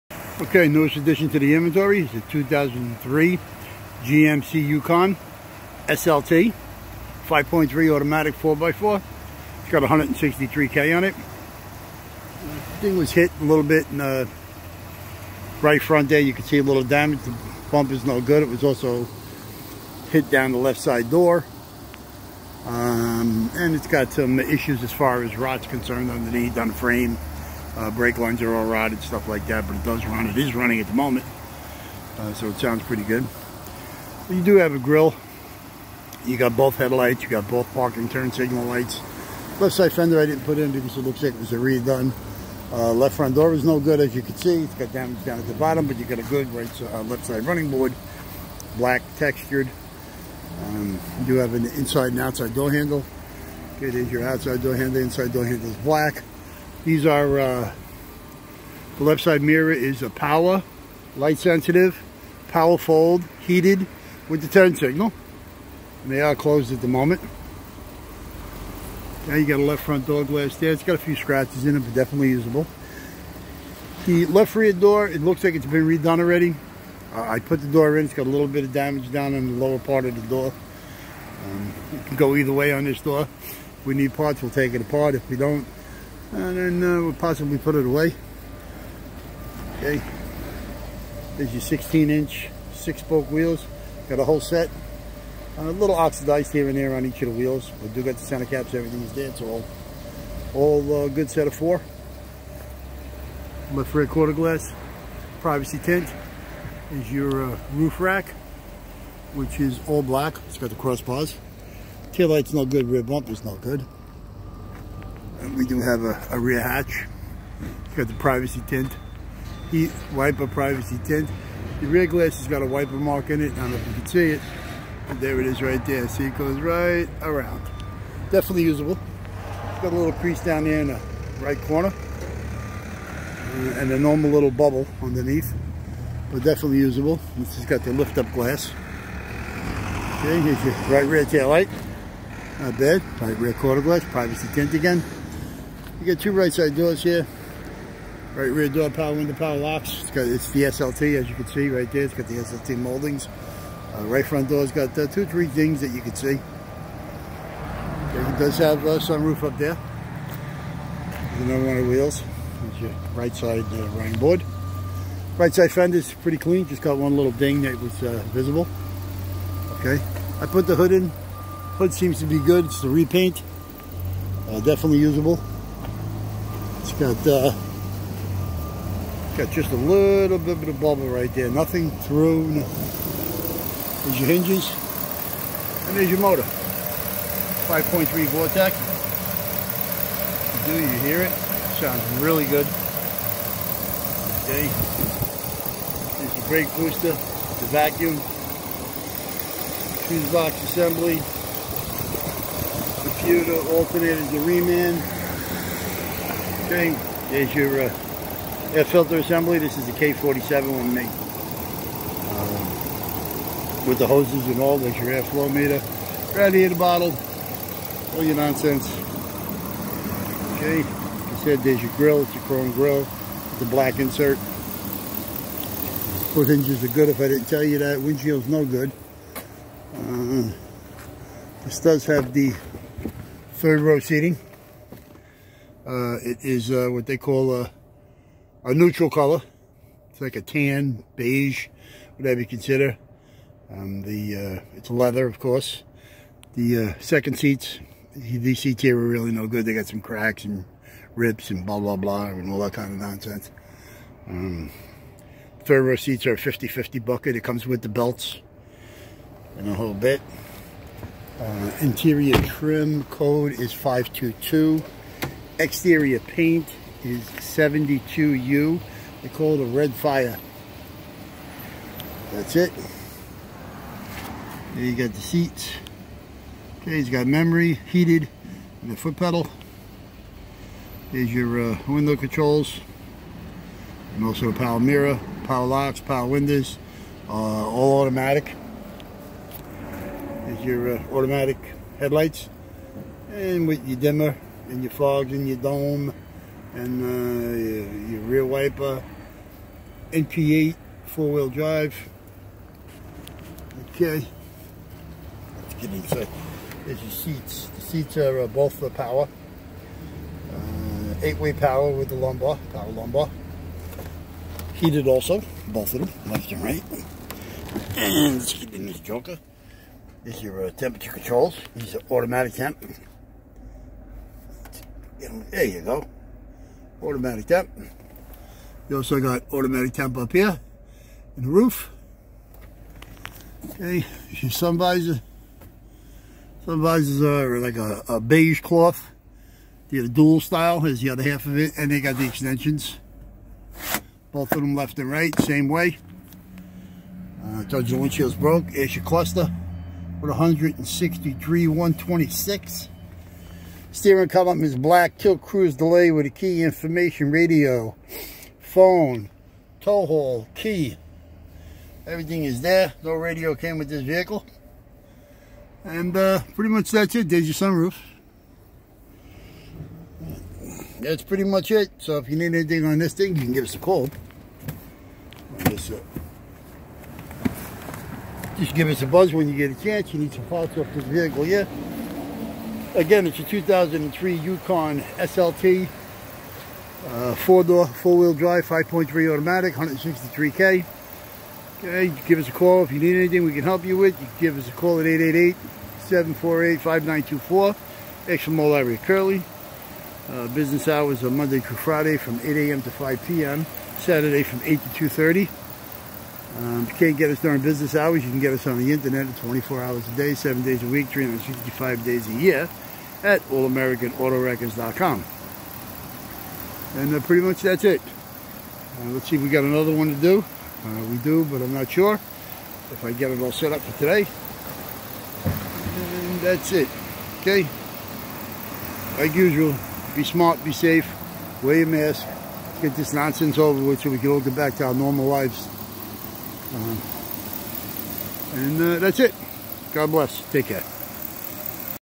All American Auto Wreckers is Parting out this 2003 Yukon SLT 4x4 5.3 At | Car Parts| Stock # 8095
This 2003 Yukon stock # 8095 runs and drives good, but no brakes. All brake lines are rotted. The Body from this 2003 GMC Yukon is in fair shape. It is a SLT Model with some nice options. The 5.3 engine runs real good and only has 163k miles on it. This 2003 Yukon also has 3.73 gear ratio in front and rear with a Posi in the rear. It also has a set of 4 16 inch alloy wheels in fair shape.  The Exterior Paint Code is 72U-Redfire and the Interior Trim Code is 522-Neutral-Tan. Also the Interior is this 2003 Yukon is Leather.. So if Moe-Larry or Curly can help you with any parts from this 2003 Yukon Stock 8095 call us Toll-Free at or go